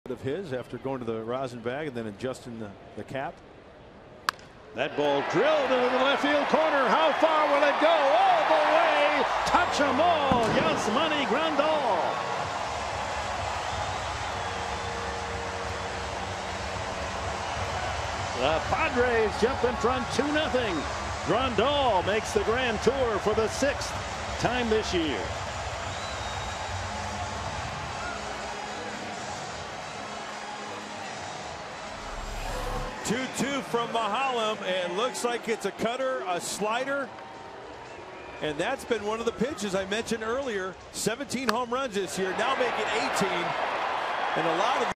2012/08/27 Grandal's two-run home run
ATL@SD: Grandal's two-run shot gives Padres a lead